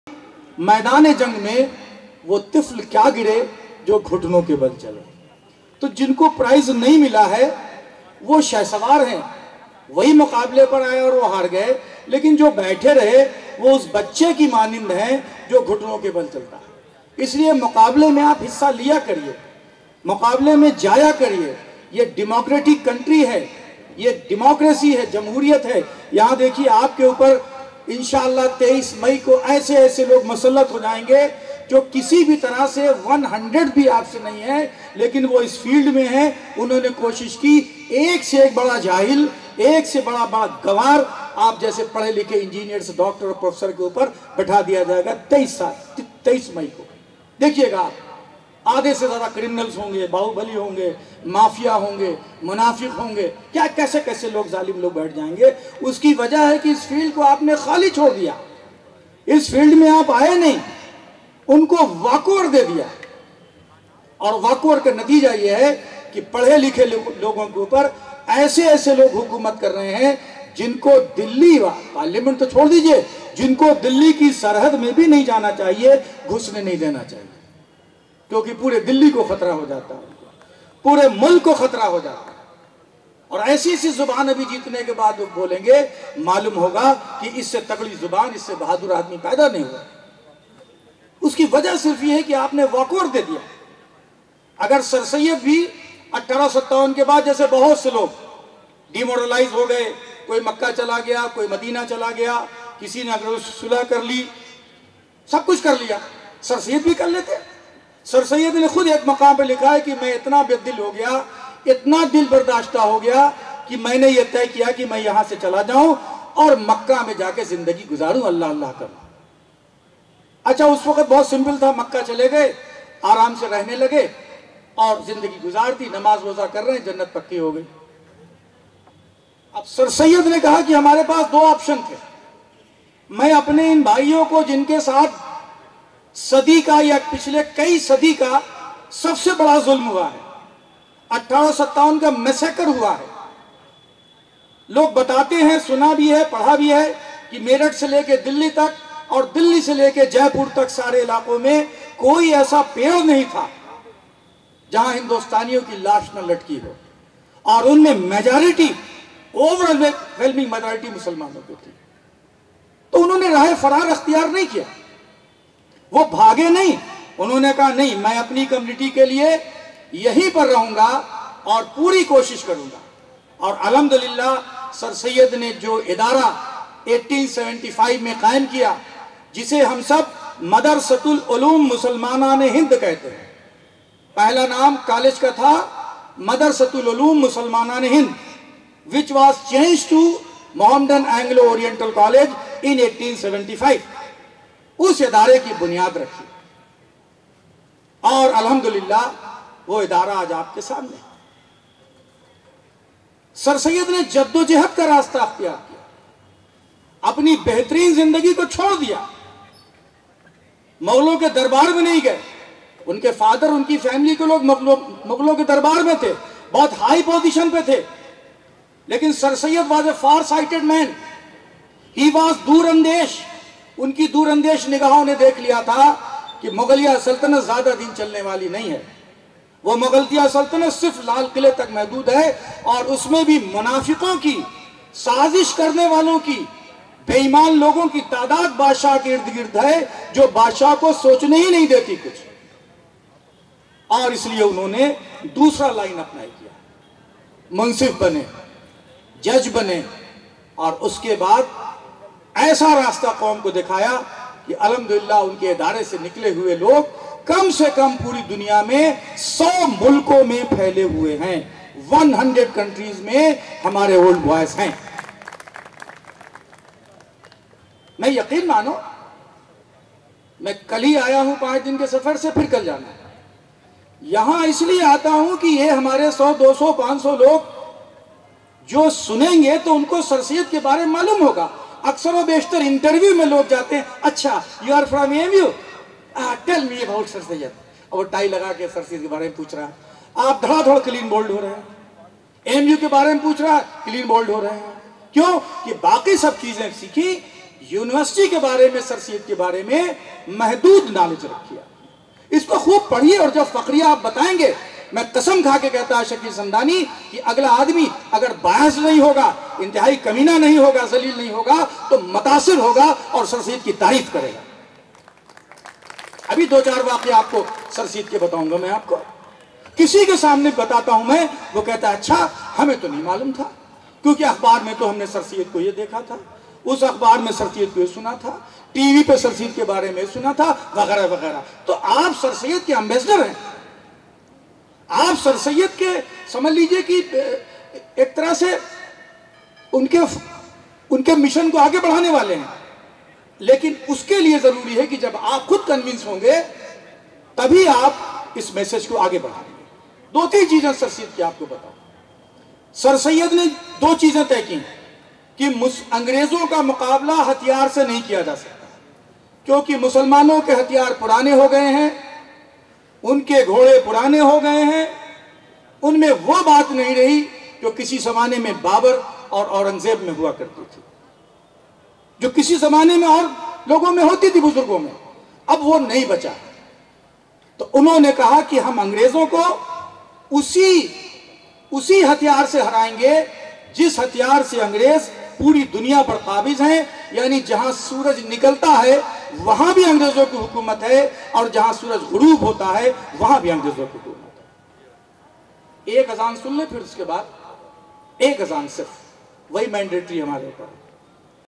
Lecture On Sir Syed Ahmed Khan | Prof.Shakeel Samdani | Pursuit | Sir Shah Sulaiman Hall
Pursuit 2019 Common Room Fest
Chief Guest  Prof Shakeel Samdani
09/04/2019